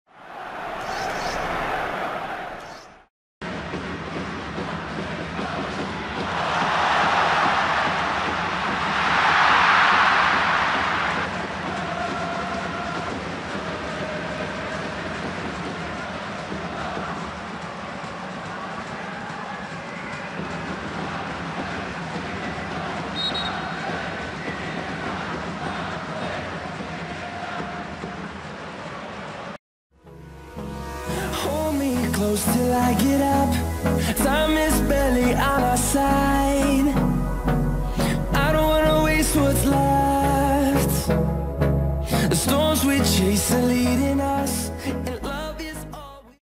El espectacular taconazo de Iniesta que provocó la ovación del estadio ⚽ JAPÓN ⚽ 2018
🔸 El manchego, que regresó a los terrenos de juego con el Vissel Kobe, dejó este taconazo precioso que levantó al público. Un gesto técnico majestuoso.

⚙ BANNER: @HypnotikGraphs